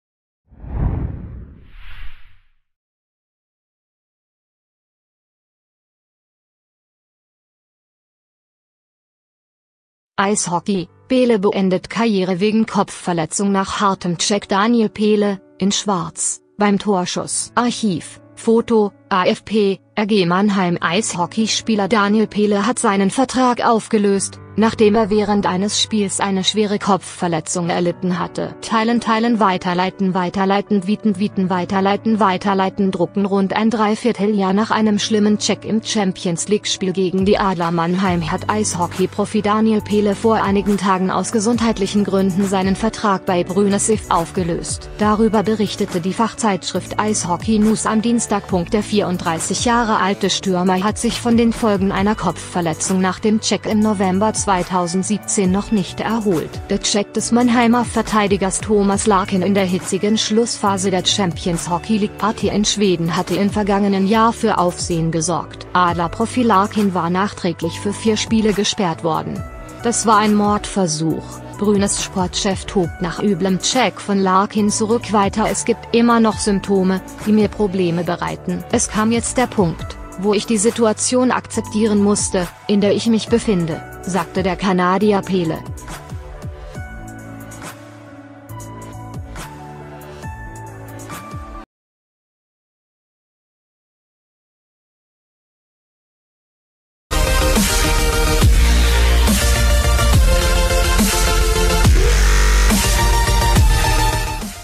Daniel Paille beendet Karriere wegen Kopfverletzung
Eishockey : Paille beendet Karriere wegen Kopfverletzung nach hartem Check Daniel Paille (in Schwarz) beim Torschuss.(Archiv) Foto: afp, RG Mannheim Eishockeyspieler Daniel Paille hat seinen Vertrag aufgelöst, nachdem er während eines Spiels eine schwere Kopfverletzung erlitten hatte.Teilen Teilen Weiterleiten Weiterleiten Tweeten Tweeten Weiterleiten Weiterleiten Drucken Rund ein Dreivierteljahr nach einem schlimmen Check im Champions-League-Spiel gegen die Adler Mannheim hat Eishockey-Profi Daniel Paille vor einigen Tagen aus gesundheitlichen Gründen seinen Vertrag bei Brynäs IF aufgelöst.Darüber berichtete die Fachzeitschrift „Eishockey News“ am Dienstag.Der 34 Jahre alte Stürmer hat sich von den Folgen einer Kopfverletzung nach dem Check im November 2017 noch nicht erholt.Der Check des Mannheimer Verteidigers Thomas Larkin in der hitzigen Schlussphase der Champions-Hockey-League-Partie in Schweden hatte im vergangenen Jahr für Aufsehen gesorgt.Adler-Profi Larkin war nachträglich für vier Spiele gesperrt worden."Das war ein Mordversuch" : Brynäs-Sportchef tobt nach üblem Check von Larkin zurück weiter „Es gibt immer noch Symptome, die mir Probleme bereiten.Es kam jetzt der Punkt, wo ich die Situation akzeptieren musste, in der ich mich befinde“, sagte der Kanadier Paille.